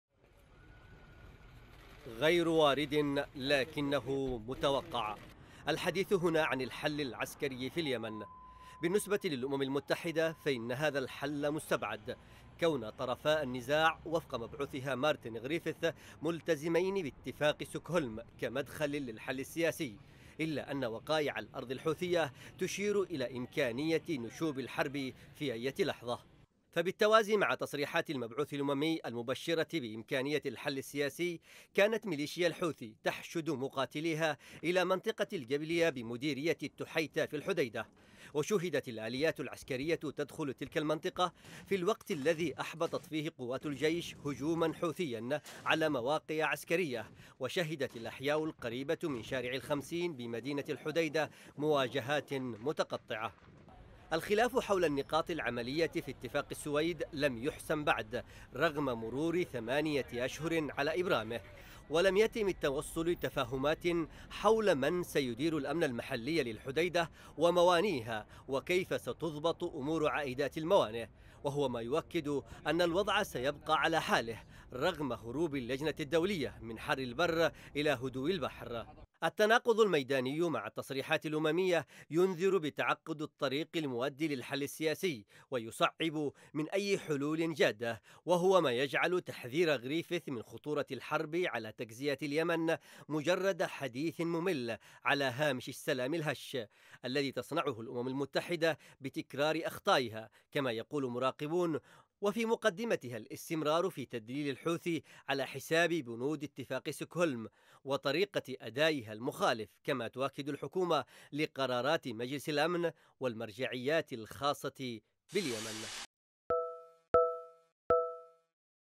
المبعوث الاممي يقول إن الحل العسكري في اليمن غير وارد وفرص السلام لا تزال معقدة|تقرير: محمد اللطيفي
سياسياً.. قال المبعوث الأممي الى اليمن مارتن غريفيث، إن الحل العسكري في اليمن غيرُ وارد.
وأوضح في مؤتمر صحفي أن طرفي النزاع ملتزمان باتفاق ستوكهولم، الذي يستغرق تنفيذه وقتاً طويلاً، مشيرًا الى أن استمرار الحرب سيعقد من حل الأزمة...